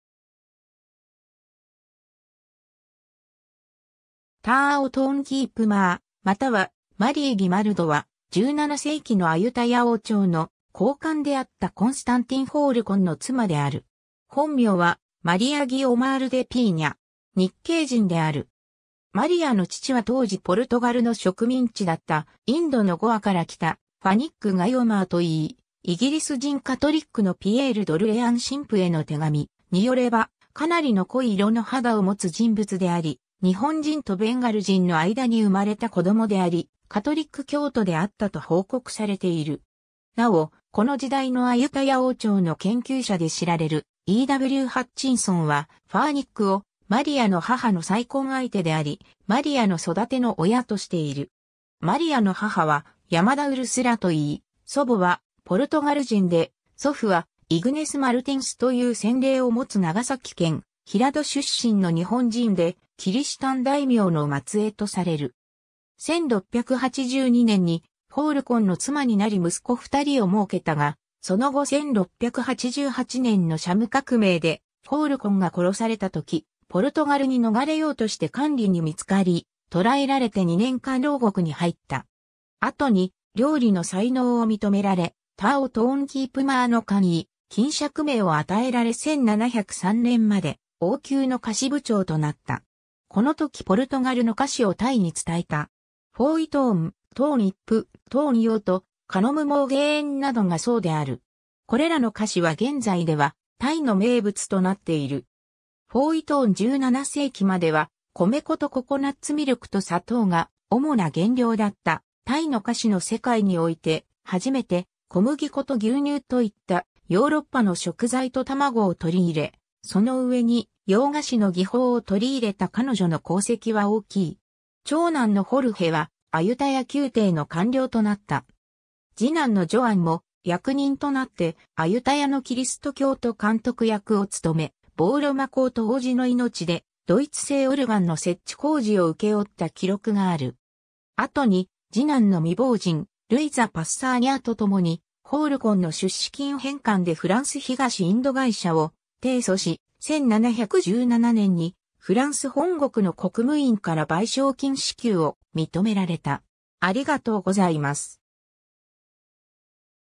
ターオ・トーンキープマー
ターオ・トーンキープマー（タイ語: ท้าวทองกีบม้า）またはマリー・ギマルド（1664年-1728年）は、17世紀のアユタヤ王朝の高官であったコンスタンティン・フォールコンの妻である。
本名はマリア・ギオマール・デ・ピーニャ（Maria Guyomar de Pinha）。
日系人である。
 マリアの父は当時ポルトガルの植民地だったインドのゴアから来たFanik Guyomar（またPhanik Guimar）と言い、『イギリス人カトリックのピエール・ドルレアン神父への手紙 (Letters of an English Catholic to Pierre d'Orleans)』によれば、かなりの濃い色の肌を持つ人物であり、日本人とベンガル人の間に生まれた子供であり、カトリック教徒であったと報告されている。
なお、この時代のアユタヤ王朝の研究者で知られるE.W.ハッチンソンはファーニックを、マリアの母の再婚相手であり、マリアの育ての親としている。
 マリアの母は山田ウルスラと言い、祖母はポルトガル人で、祖父はイグネス・マルティンス (Ignez Martinz) という洗礼を持つ長崎県平戸出身の日本人で、キリシタン大名（大友氏であると言われる）の末裔とされる。
 1682年にフォールコンの妻になり息子2人を儲けたが、その後1688年のシャム革命でフォールコンが殺されたときポルトガルに逃れようとして官吏に見つかり、捕らえられて2年間牢獄に入った。
後に料理の才能を認められ、ターオ・トーンキープマーの官位・欽錫名を与えられ1703年まで王宮の菓子部長となった。
 このときポルトガルの菓子をタイに伝えた。
フォーイ・トーン（ฝอยทอง、日本では鶏卵素麺と呼ばれる）、トーン・イップ（ทองหยิบ、卵黄を砂糖で煮て花の形を付けた菓子）、トーン・ヨート（ทองหยอด;、前述の菓子の形を付けたもの）、カノムモーゲーンなどがそうである。
これらの菓子は現在ではタイの名物となっている。
 フォーイ・トーン 17世紀までは米粉とココナッツミルクと砂糖が主な原料だったタイの菓子の世界において、初めて小麦粉と牛乳といったヨーロッパの食材と玉子を取り入れ、その上に洋菓子の技法を取り入れた彼女の功績は大きい。
 長男のホルヘはアユタヤ宮廷の官僚となった。
次男のジョアンも役人となってアユタヤのキリスト教徒監督役を務め、ボーロマコート王子の命でドイツ製オルガンの設置工事を請け負った記録がある。
後に次男の未亡人ルイザ・パッサーニャと共に、フォールコンの出資金返還でフランス東インド会社を提訴し、1717年にフランス本国の国務院から賠償金支給を認められた。